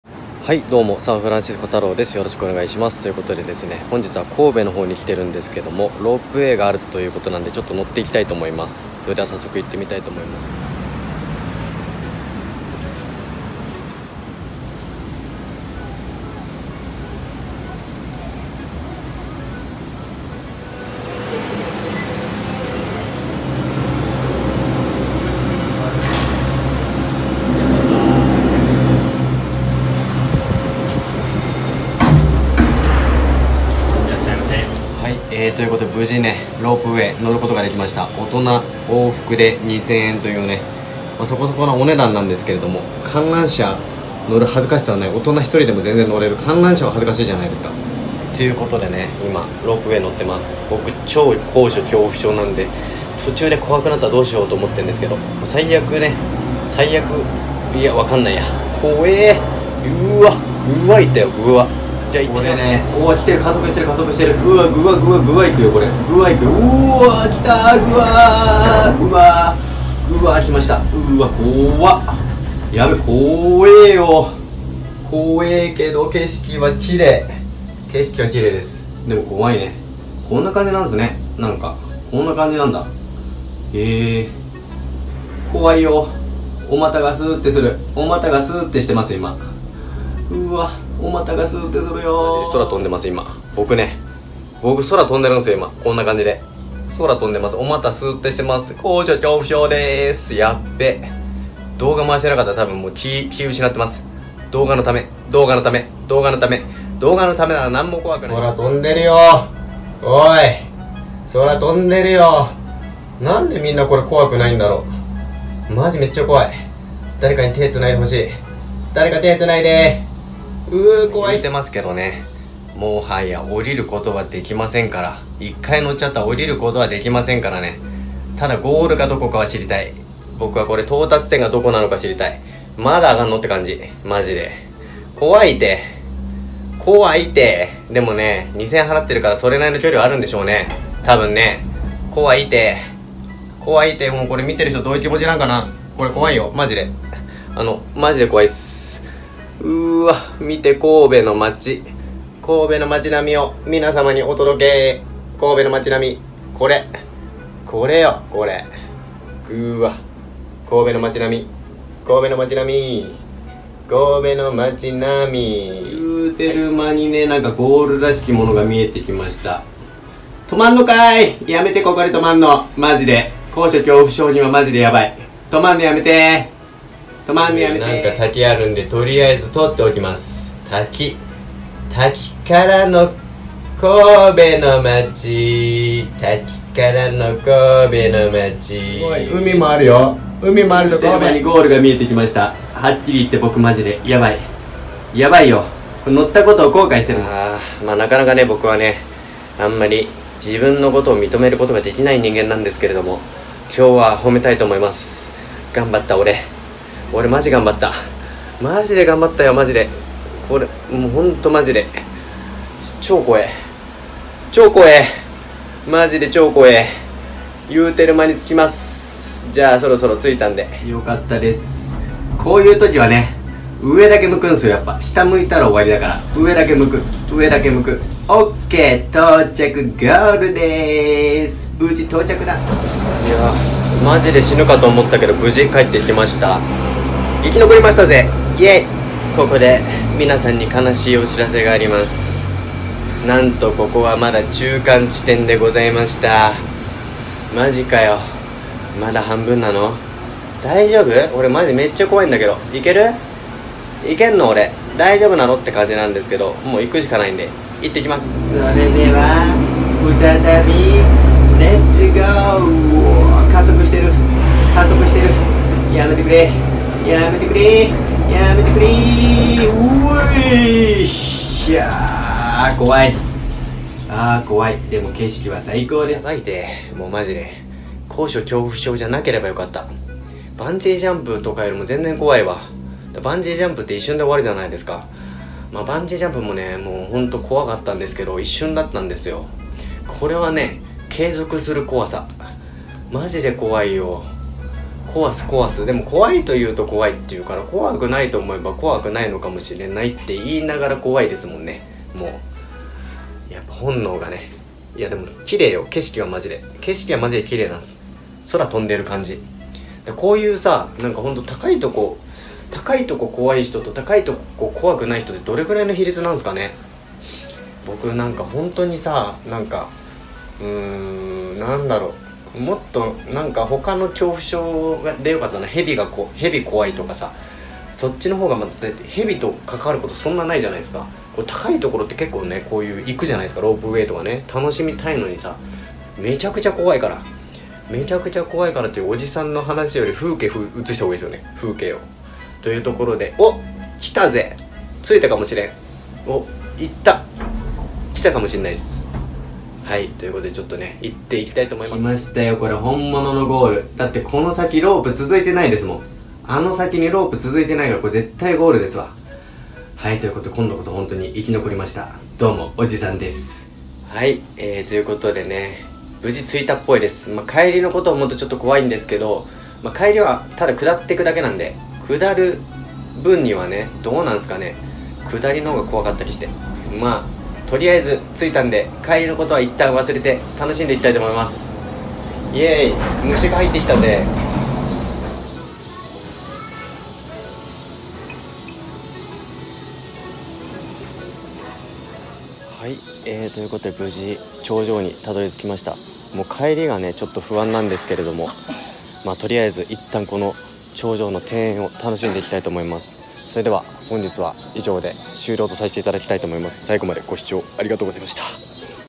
高所恐怖症のおじさんロープウェイに乗る。
高所恐怖症のおじさんロープウェイにのる。